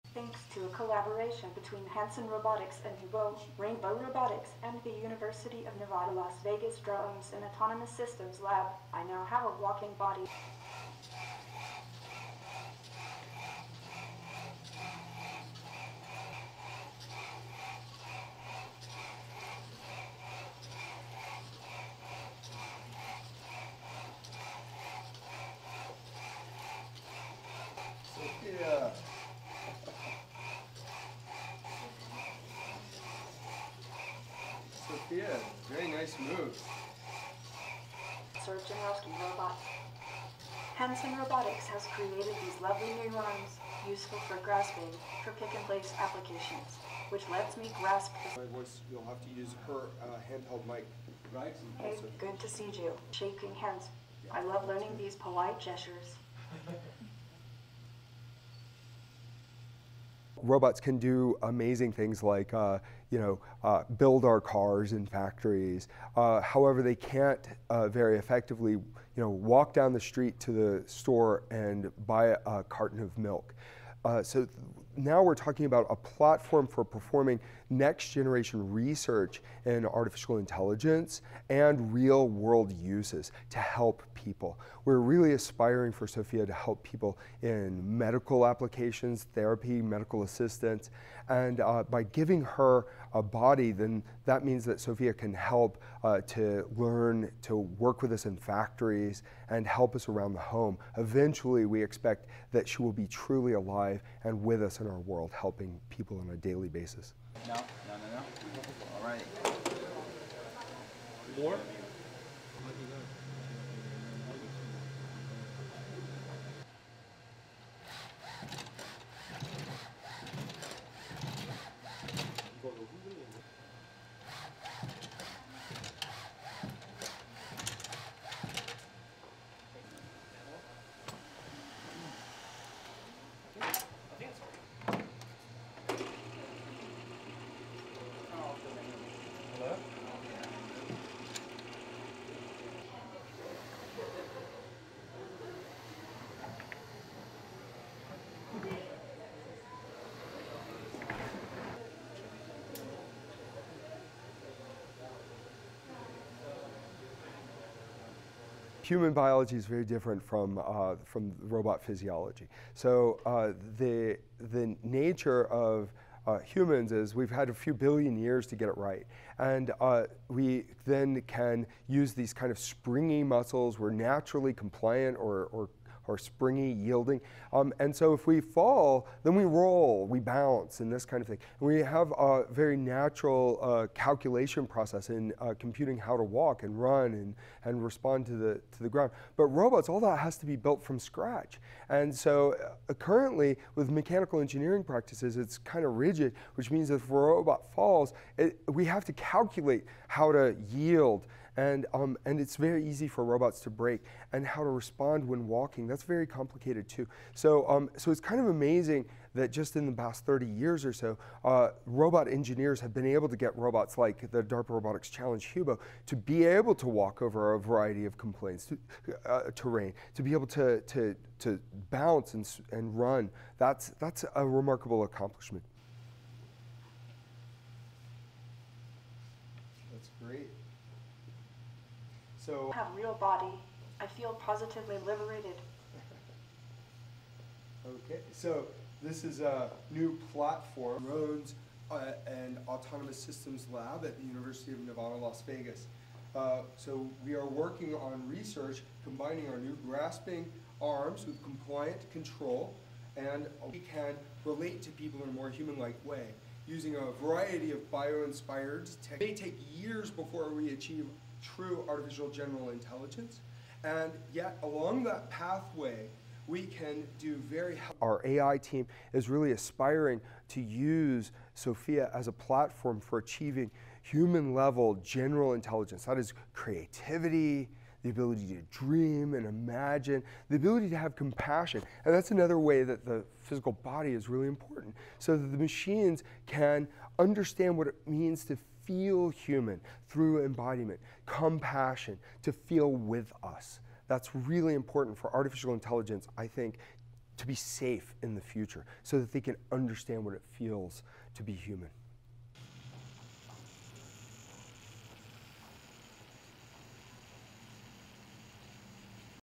Sophia robot takes first steps ++EMBARGOED 1300 GMT++
(8 Jan 2018) LEADIN:
The humanoid robot Sophia is taking her first steps thanks to a new partnership with a university robotics lab.
The Hong Kong-based startup Hanson Robotics has joined forces with experts at the University of Las Vegas to give walking legs to the talkative droid.
STORYLINE:
One small step for Sophia, one giant leap for AI.
In the workplace of the Drones and Autonomous Systems Lab (DASL) at the University of Las Vegas, Sophia is taking her first ever steps.
Gingerly, she shuffles along watched closely by her creator, Hanson Robotics founder and CEO David Hanson.
The addition of legs to the lifelike bot is another stage in pushing the boundaries of artificial intelligence for Hanson and his team.
"We're really aspiring for Sophia to help people in medical applications, therapy, medical assistance and by giving her a body then that means that Sophia can help to learn to work with us in factories and help us around the home," says Hanson.
"Eventually, we expect that she will be truly alive and with us in our world helping people on a daily basis."
Experts from the University of Las Vegas created DRC-HUBO, a walking robot that won the DARPA Robotics Challenge (DRC) in 2015, beating 22 competitors from five different countries.
That expertise is now being applied to Sophia's evolving mobility. Hanson Robotics is also partnering with South Korea based Rainbow Robotics to make Sophia mobile.
"So currently, with mechanical engineering practices, it's kind of rigid, which means a if robot falls, we have to calculate how to yield and it's very easy for robots to break and how to respond when walking that's very complicated too," says Hanson.
Sophia first debuted in March 2016.
A multinational team of scientists and engineers are fine tuning her appearance and the algorithms that enable her to smile, blink and refine her understanding and communication.
Her skin is made of a nanotech material that Hanson invented and dubbed "Frubber," short for flesh-rubber, that has a flesh-like bouncy texture mimicking human skin.
Cameras in her eyes and a 3D sensor in her chest help her "see."
The processor that serves as her brain combines facial and speech recognition, natural language processing, speech synthesis and a motion control system.
"Our AI team is really aspiring to use Sofia as a platform for achieving human-level general intelligence, that's creativity, the ability to dream and imagine, the ability to have compassion," says Hanson.
"And that's another way that the physical body is really important. So, the machines can understand what it means to feel human through embodiment, compassion, to feel with us.
"That's really important for artificial intelligence, I think, to be safe in the future, so they can understand what it feels to be human."
The annual Consumer Electronics Show (CES) runs 9-12 January.
Over 3,900 exhibitors will crowd Las Vegas convention centres showcasing the latest in high-tech products, devices and services.
Top trends at this year's event include artificial intelligence, the 'Internet of Things' and mobility tech.